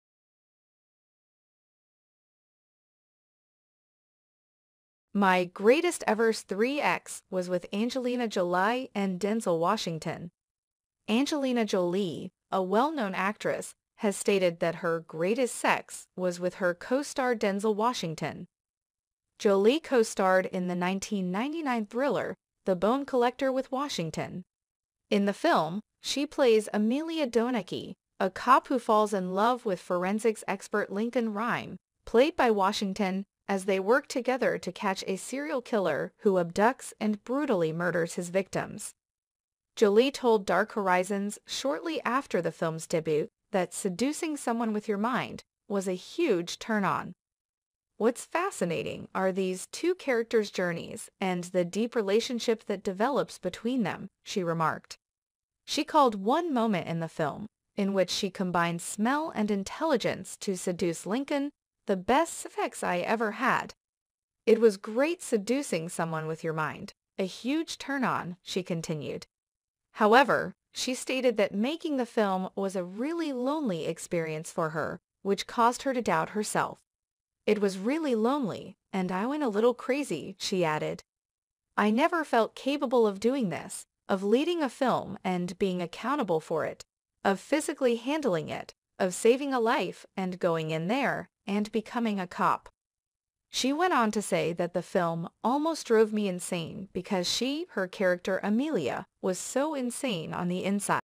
My ‘best ever s€x’ was with Denzel Washington — Angelina Jolie.
My ‘best ever s€x’ was with Denzel Washington — Angelina Jolie.Speaking to Dark Horizons shortly after the film’s release, Jolie said that “seducing someone with your mind” was a “huge turn-on.”

“What’s fascinating are the journeys these two characters undertake, and the deep relationship that evolves between them,” she said.Referring to one scene in the film when she uses smell and intellect to seduce Lin8coln, she said this was the “best sex I ever had.”It was great seducing someone with your mind, a huge turn-on,” she added.

But she said shooting the film was a “really lonely” experience for her and led her to doubt herself.

She explained: “It was really lonely and I went a bit nuts. From the beginning, I felt as if I was not capable of doing this, of leading a film and being responsible for this, of physically handling this, of saving a life and going in there and being a cop.”

She added that the film “nearly drove me insane because she [her character Amelia] was so insane inside

In Russian language

Мой "лучший секс" был с Дензелом Вашингтоном - Анджелина Джоли. В интервью Dark Horizons, данном непосредственно после выхода фильма, Джоли заявила, что "соблазнение кого-то своим умом" - это "огромный возбудитель".

"Интересными являются путешествия, которые предпринимают эти два персонажа, и глубокие отношения, которые развиваются между ними", - сказала она. Относительно одной из сцен фильма, когда она использует запах и интеллект, чтобы соблазнить Линкольна, она сказала, что это "лучший секс, который у нее был". "Это было потрясающее соблазнение кого-то своим умом, огромный возбудитель", - добавила она.

Однако она сказала, что съемки фильма были для нее "очень одиноким" опытом, который заставил ее сомневаться в себе.

Она объяснила: "Мне было очень одиноко, и я слегка схожу с ума. С самого начала я чувствовала, что не способна справиться с этим, быть ответственной за фильм и спасать жизни, быть полицейским. Фильм почти свел меня с ума, потому что она [ее персонаж Амелия] была настолько безумной внутри"